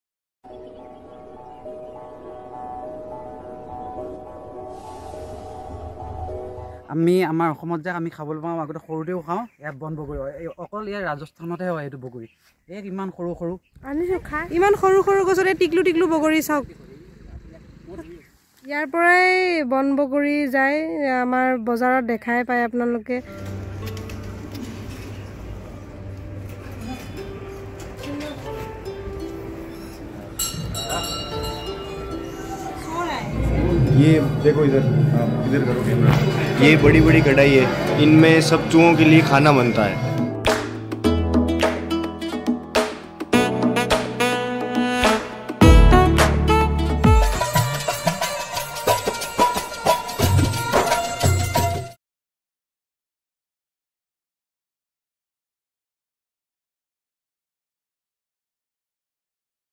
Explore Karni Mata Mandir Rajasthan | Coming Soon | Rajasthan Vlog Part 4 | Sagarika's Vlog Talk
Explore Karni Mata Mandir Bikaner, Rajasthan, India